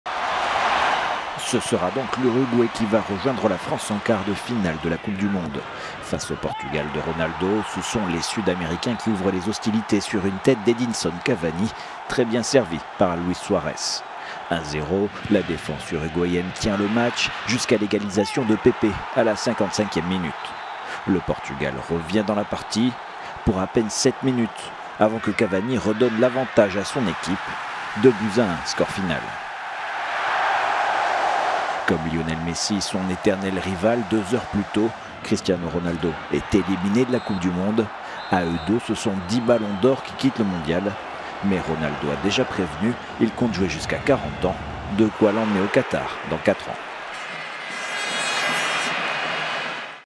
Le Portugal de Ronaldo éliminé
Le Portugal éliminé, la France affrontera l'Uruguay au prochain tour.…